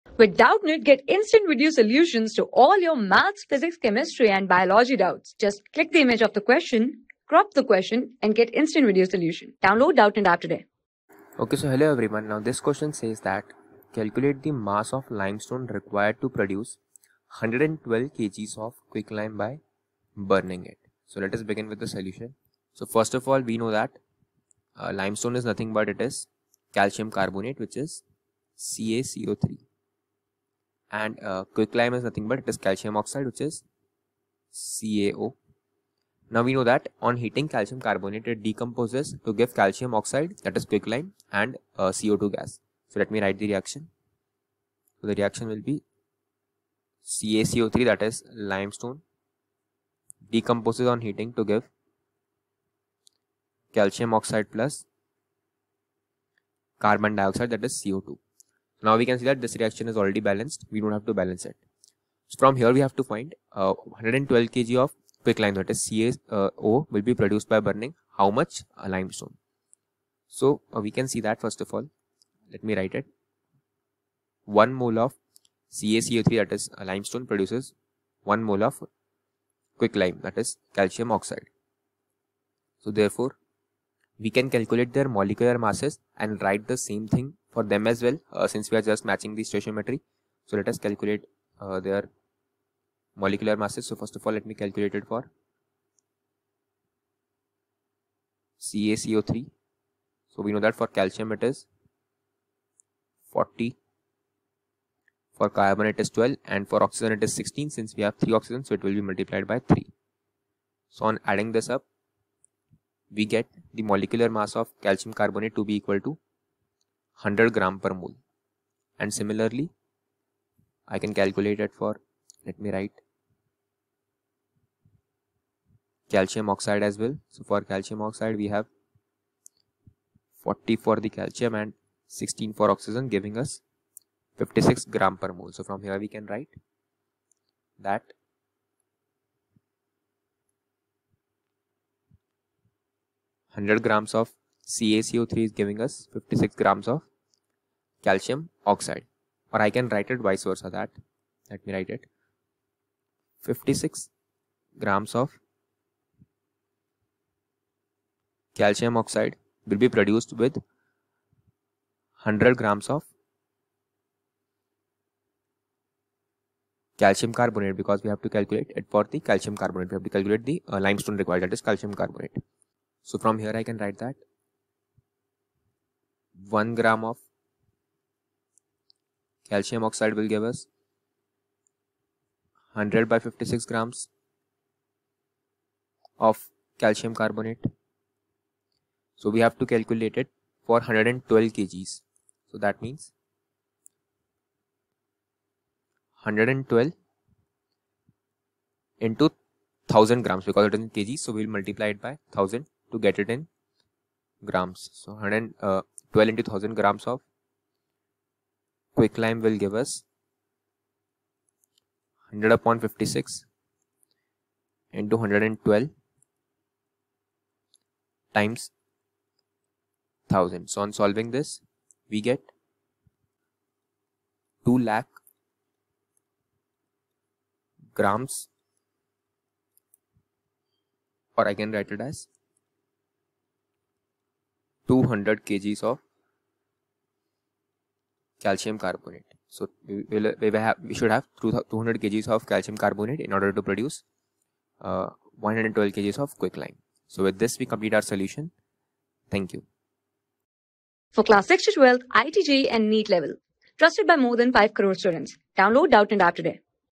Calculate the mass of limestone required to produce 112 kg of quicklime by burning it. | 10 | S...
Calculate the mass of limestone required to produce 112 kg of quicklime by burning it.
Class: 10
Subject: CHEMISTRY
Chapter: SELF ASSESSMENT PAPER 2
Board:ICSE

👉 Have Any Query? Ask Us.